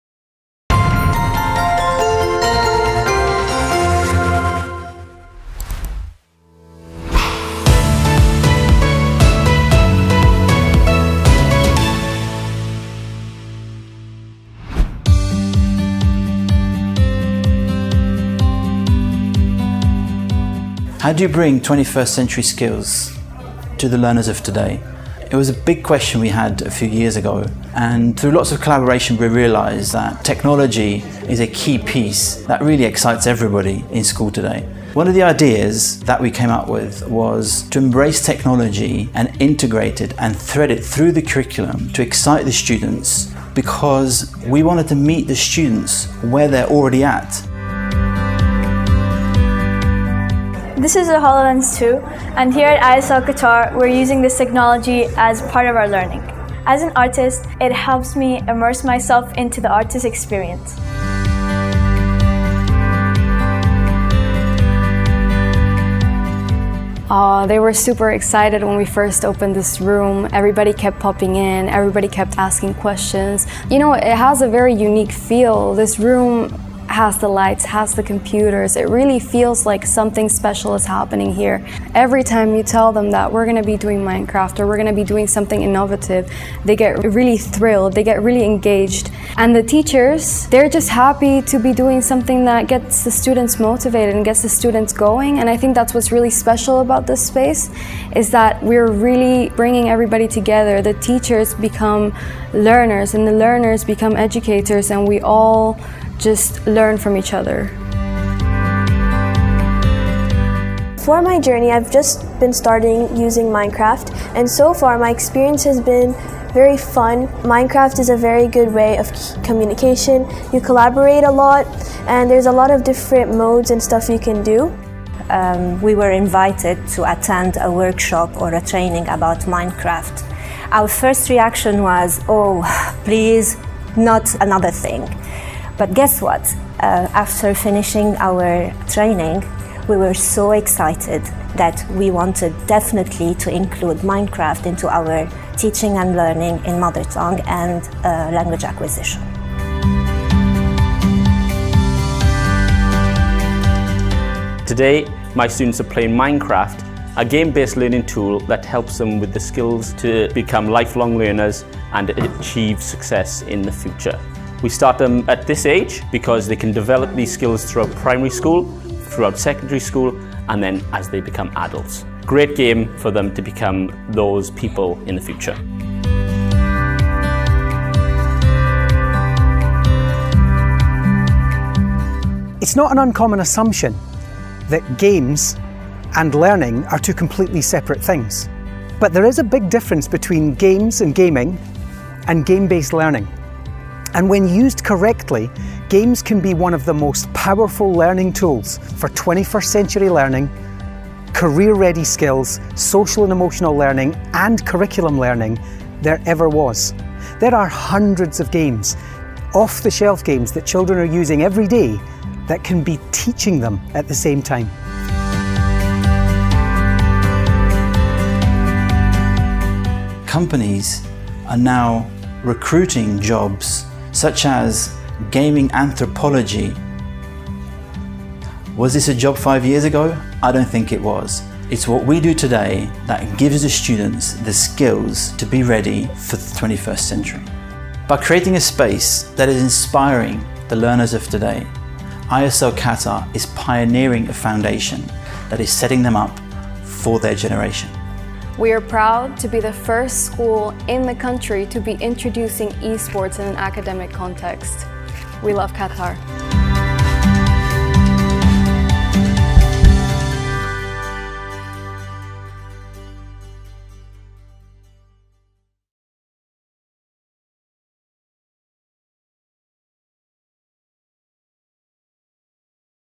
Know how this school is adopting Game-Based Learning | Trending In Qatar
In this video, learn how the International School of London (ISL) in Qatar is embracing technology to teach students key 21st-century skills.

Did you know that game-based learning has proven to excite the students and make them better learners, collaborators, and thinkers?!

What other trending topics in Qatar should we cover?

Doha, Qatar